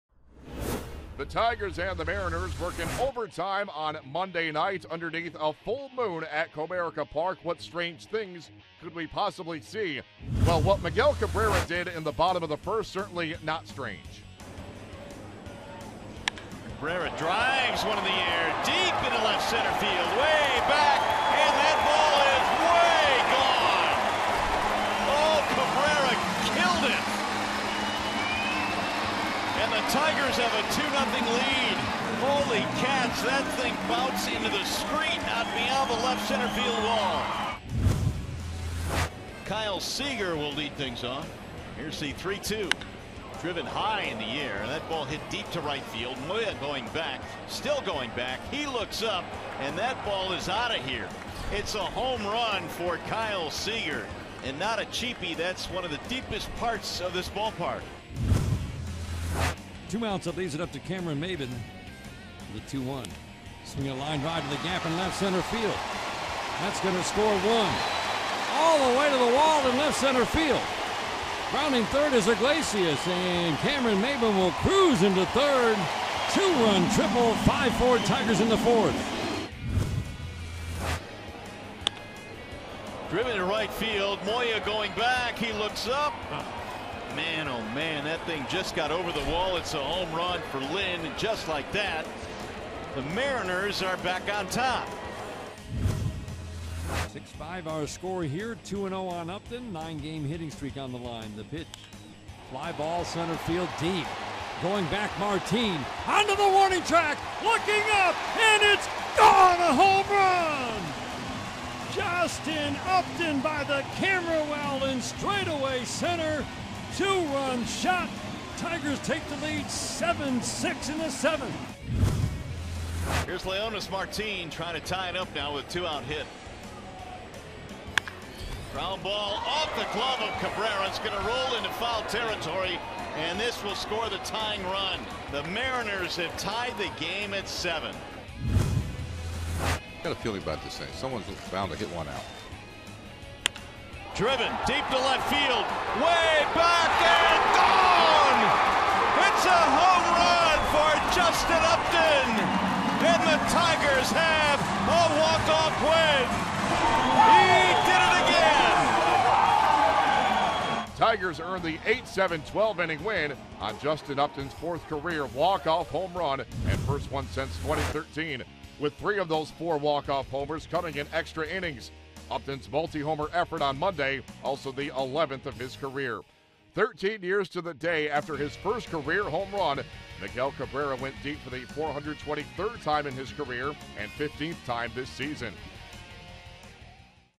6/20/16: Upton's walk-off lifts Tigers to 8-7 win
Daily Recap: Miguel Cabrera hit a moonshot and Justin Upton hit two homers, including a walk-off smash in the 12th in the Tigers' 8-7 win